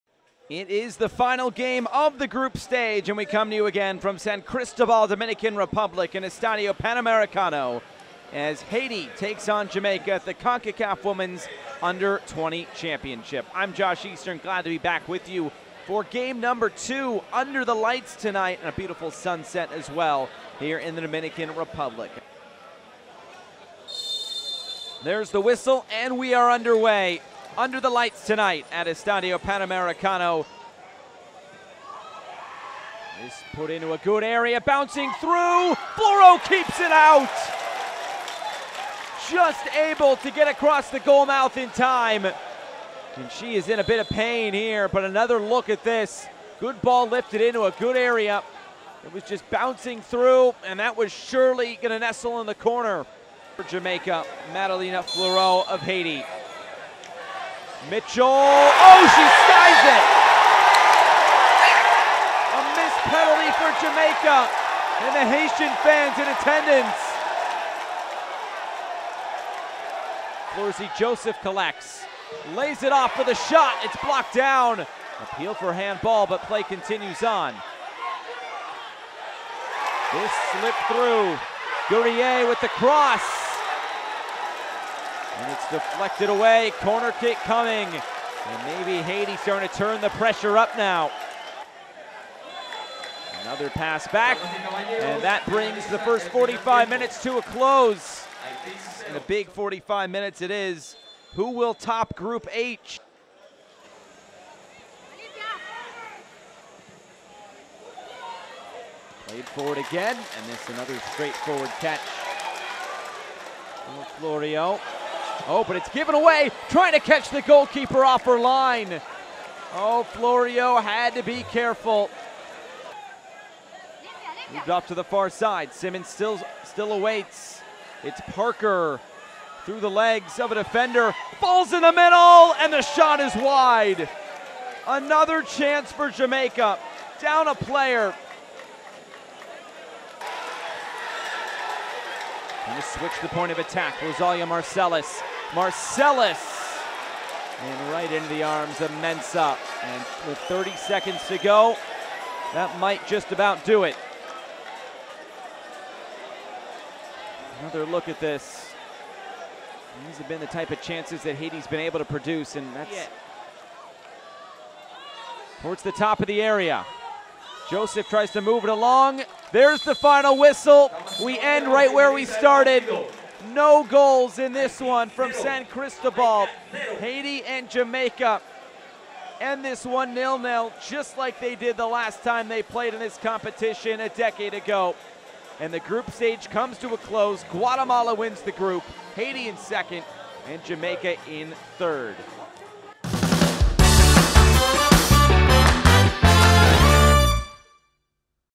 Concacaf Womens Under-20 Championship | 2022 Highlights: Haiti vs Jamaica
Highlights of the game 📹

In a very closed game with Jamaica 🇯🇲, Haiti 🇭🇹 obtains a place in CWU20 round of 16 🥳